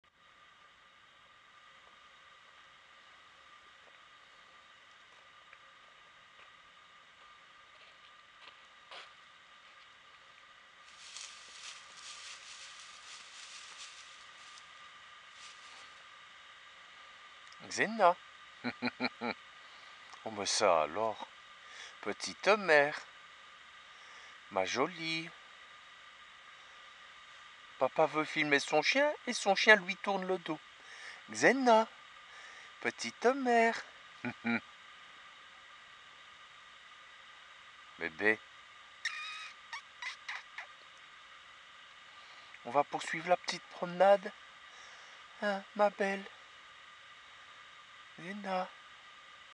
Je tourne le dos à Papounet
Comme pour faire la nique à Papounet, jolie Xéna lui tourne le dos alors qu'il essaie de filmer sa fille à proximité de la haie cernant le nouveau cimetière de Jeumont ...!

Document réalisé le 5 décembre 2016